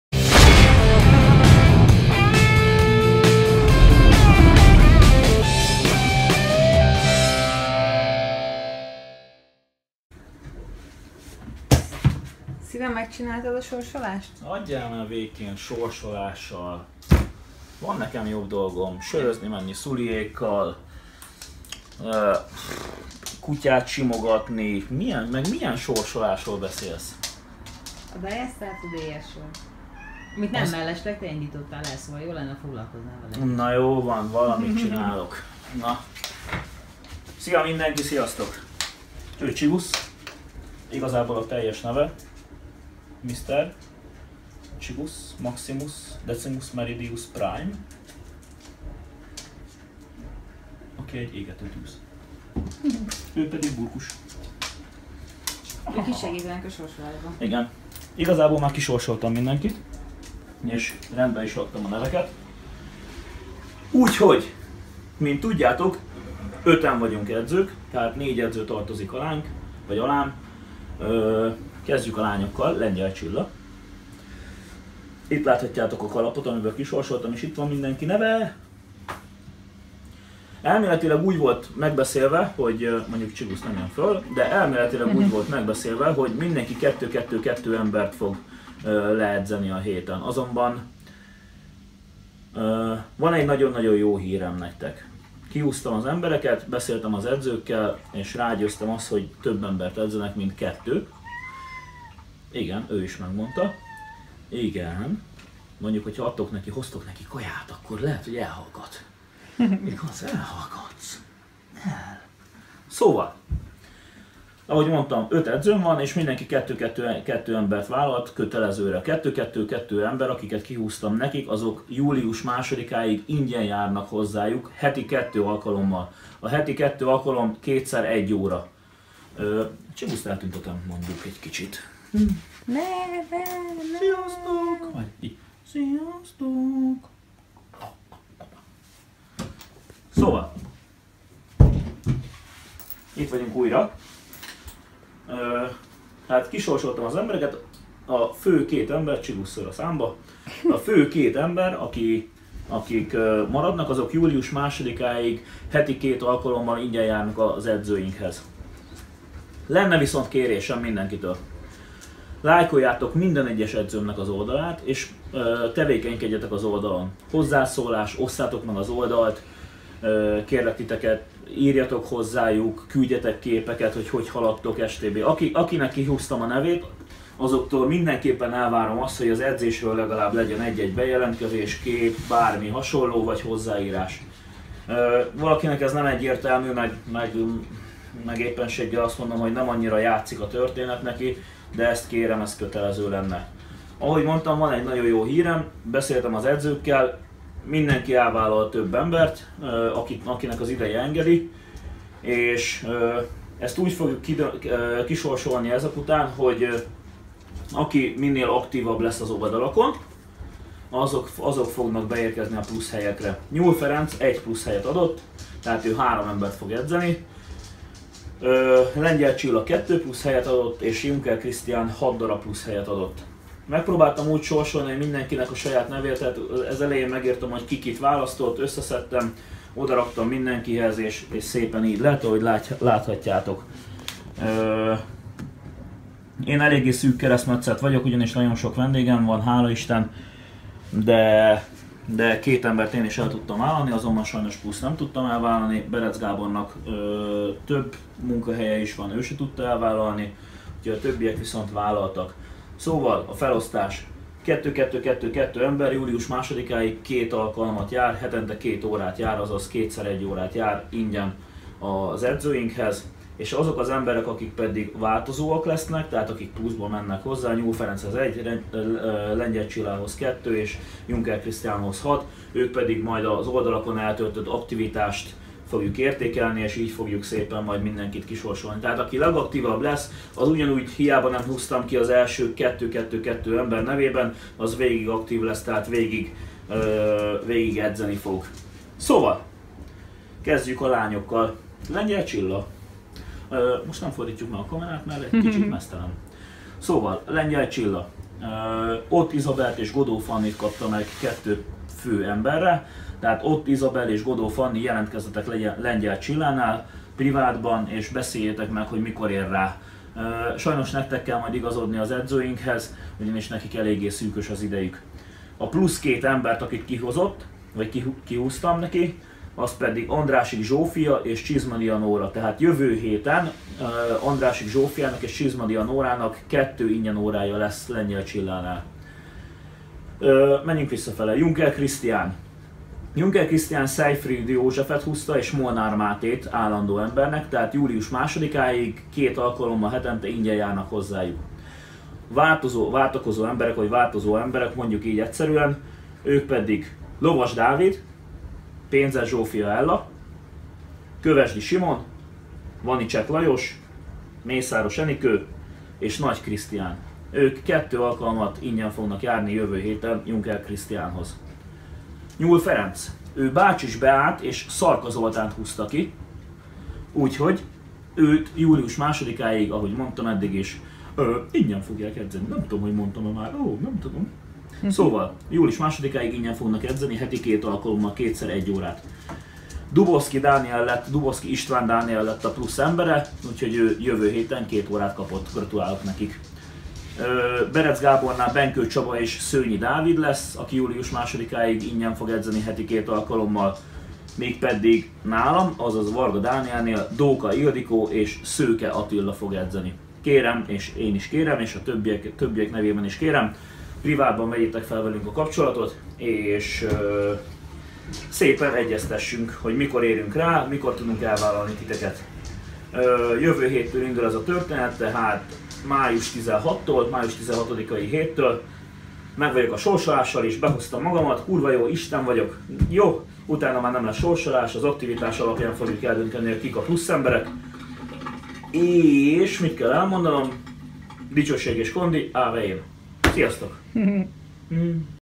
Diet Start Today Sorsolás
Ez a kis film, most nem a gyakorlatokról, hanem a Diet Start Today nevezetű rendezvény sorsolásáról szól, mivel ingyen vállaltunk edzéseket az edzőimmel. Gratulálunk a nyerteseknek, s jó edzést kívánok nektek!